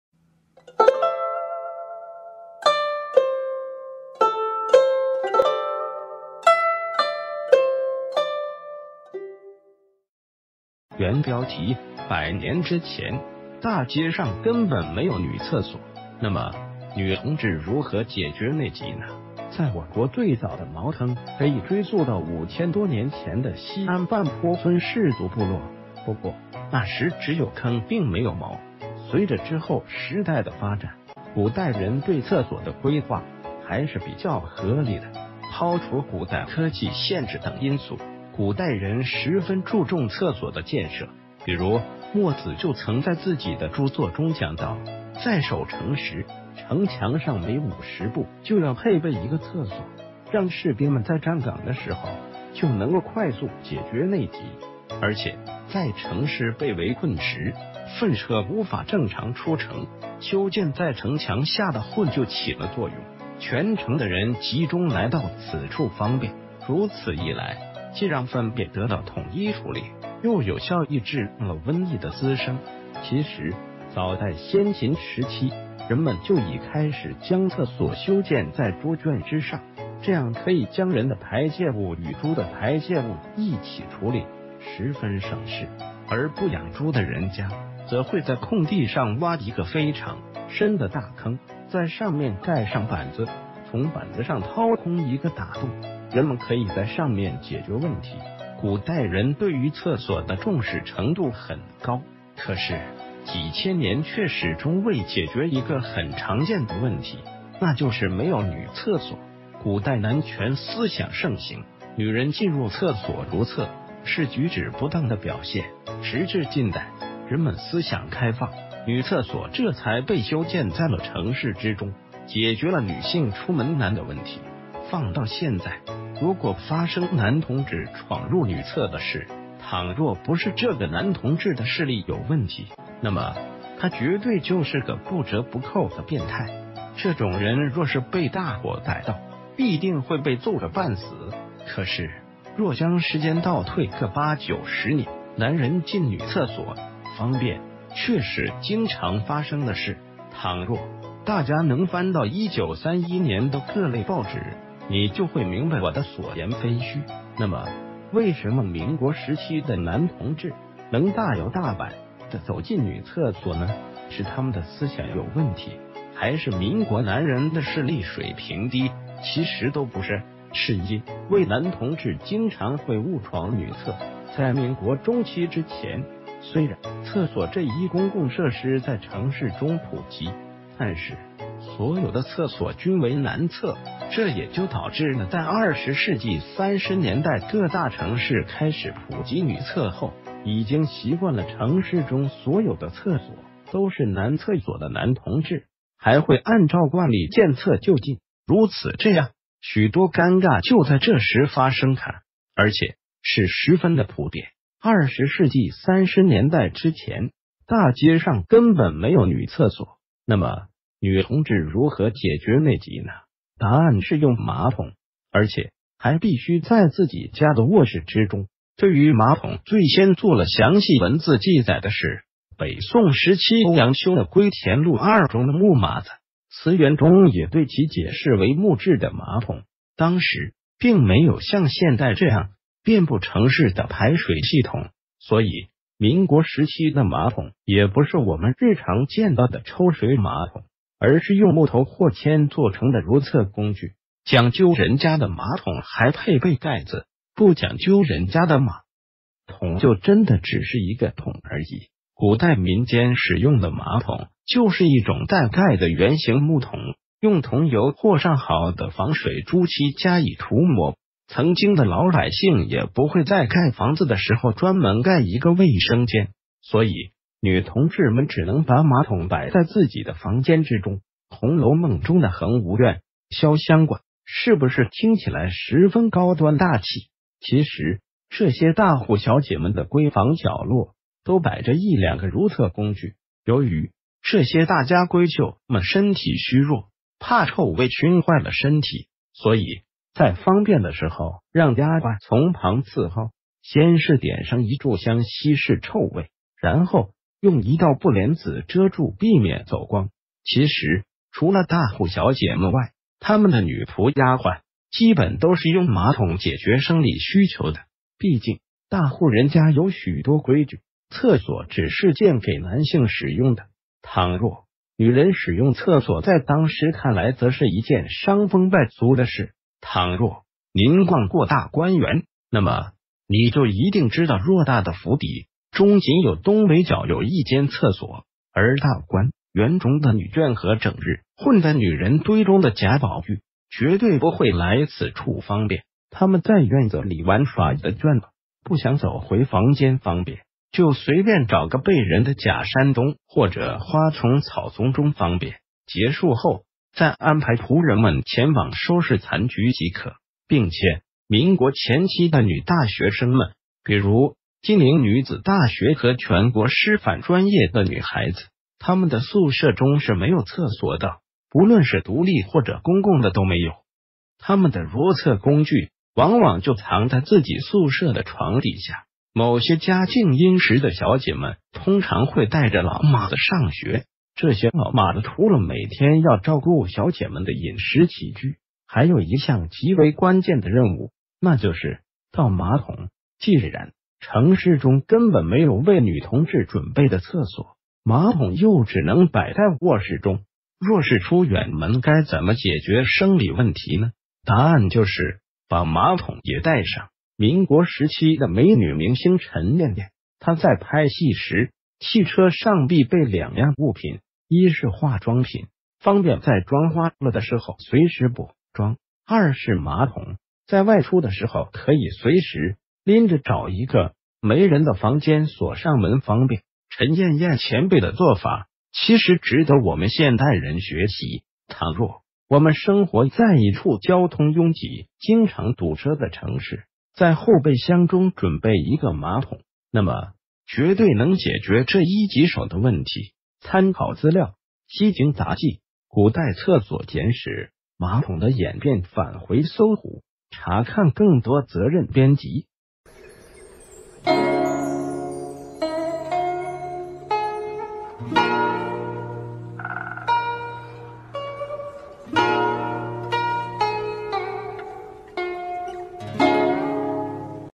百年之前，大街上根本沒有女廁所，那么，女同志如何解決內急呢？_馬桶
原標題：百年之前，大...